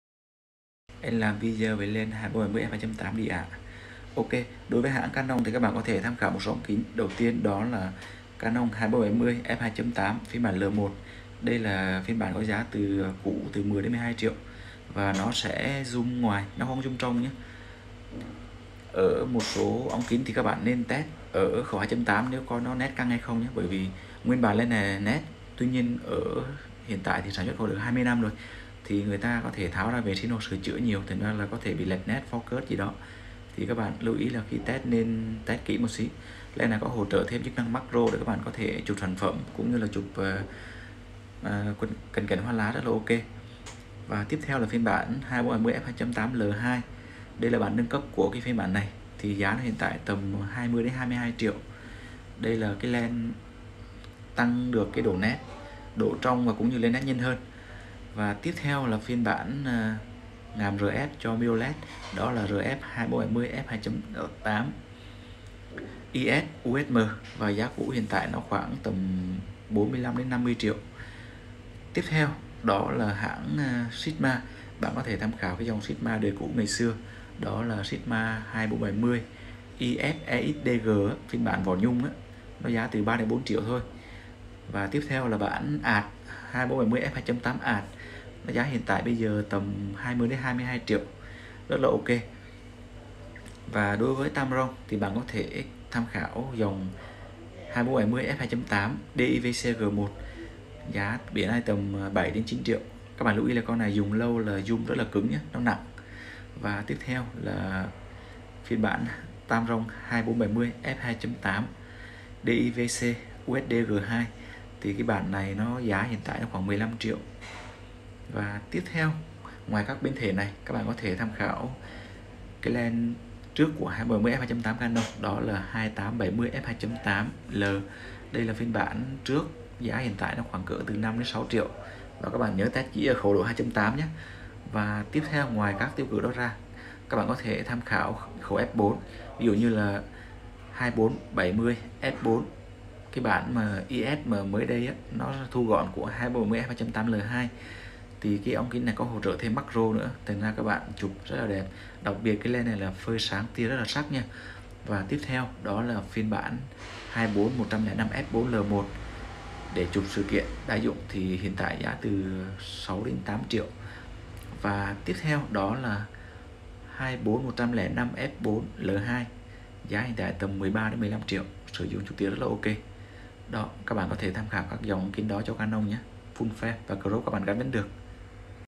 Các ống kính tiêu cự 24-70F2.8 sử dụng cho máy ảnh Canon Fullframe
Các ống kính tiêu cự 24-70F2.8 sử dụng cho máy ảnh Canon Fullframe DSLR và Mirrorless, chi tiết các bạn theo dõi video nhé.
📲Theo dõi Admin để xem các cập nhật mới nhất về bài hướng dẫn và chia sẻ tài nguyên tại:
📌Hotline mua máy cũ: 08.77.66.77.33